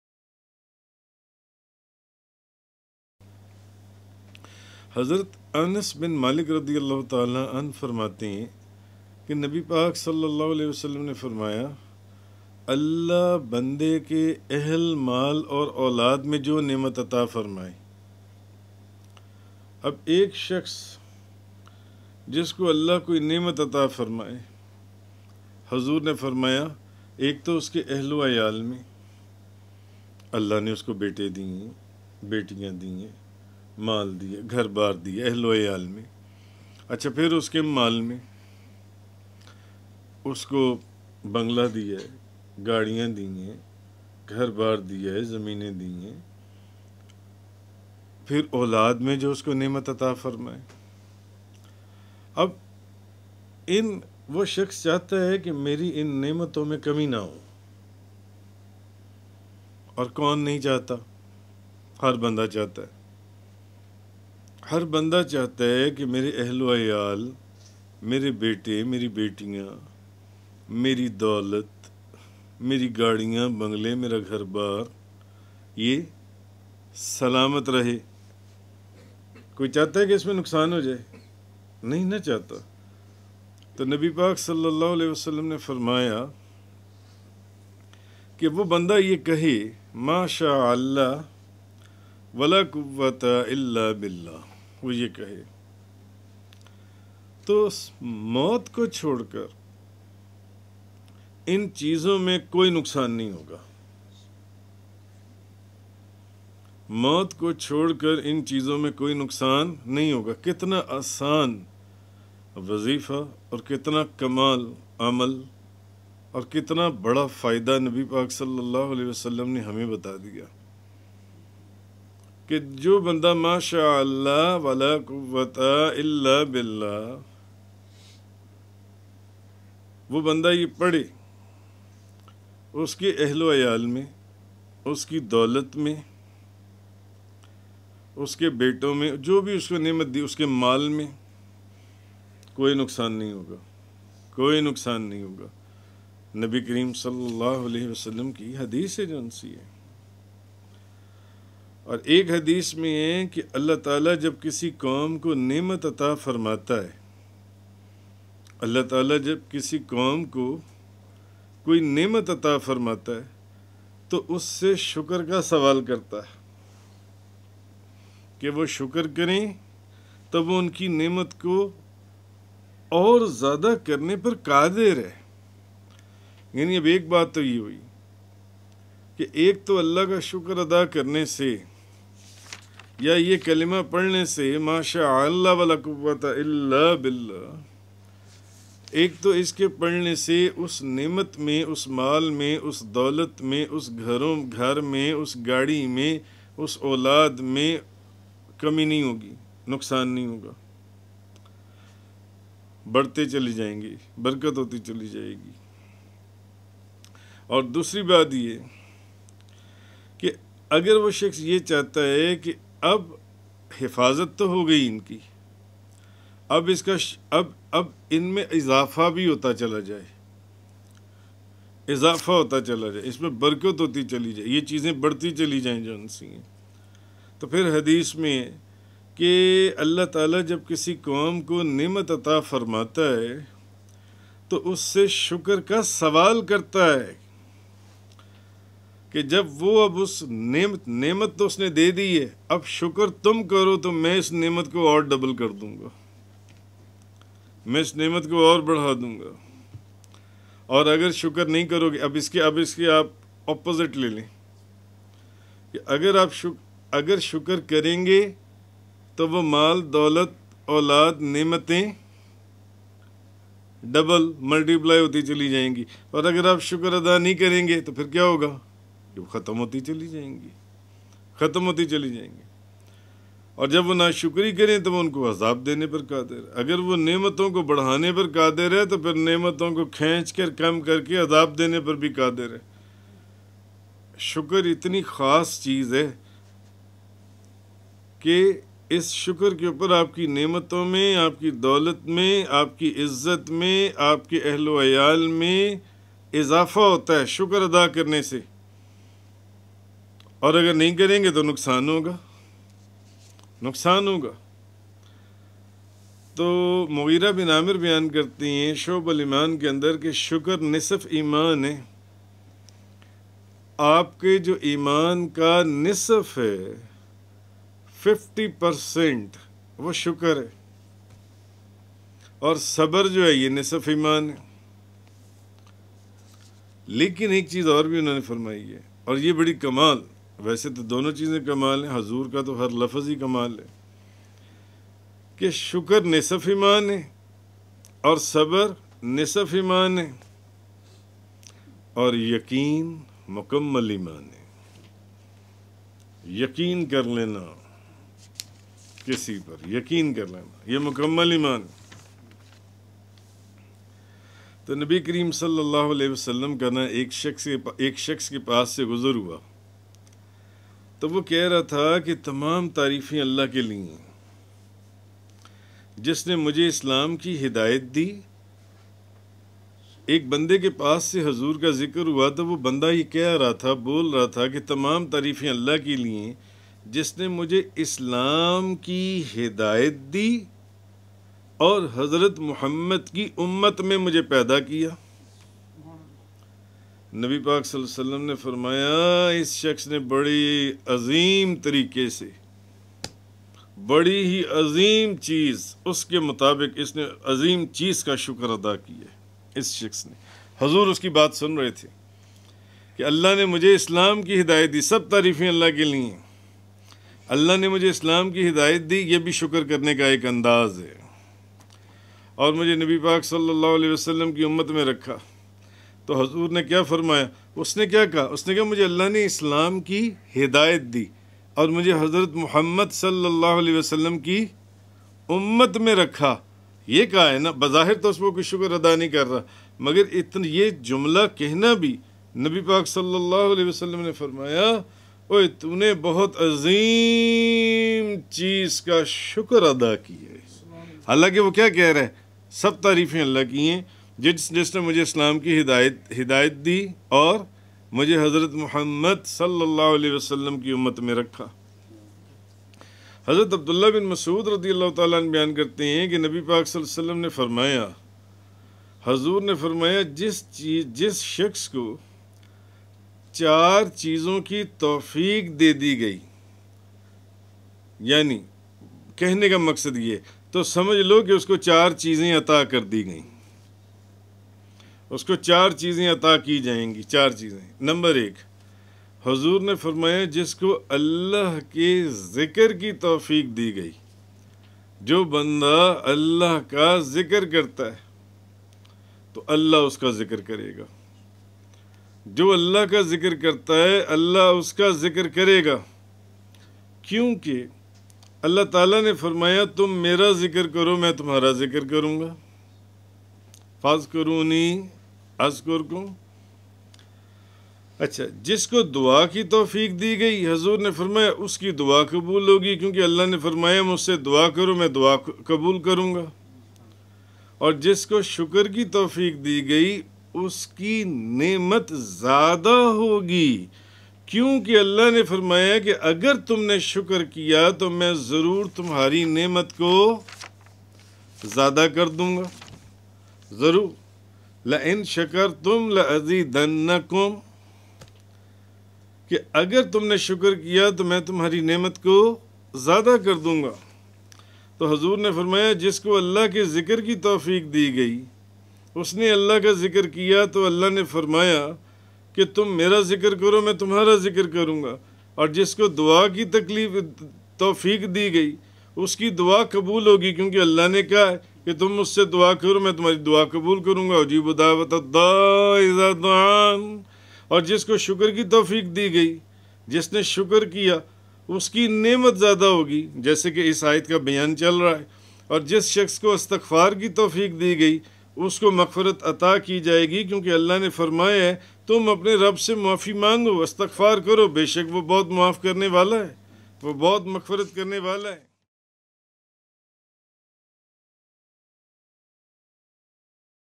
Shuker Sy Neamtoo Ko Qaid Kr Lo | Shaykh Muhammad Mohsin Munawar Yousafi | Naqshbandi Mohsini
0:05 حضرت انس بن مالک
1:24 نقصان سے بچنے کا وظیفہ
3:30  شکر کرنے سے مال و دولت میں اظافہ
7:33 چار چیزوں کی توفیق

Clip # 00 Dated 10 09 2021 A Clip II Sura 14 aya 7 8 SHuker Ada kernay waloun ki doolat aur naimatou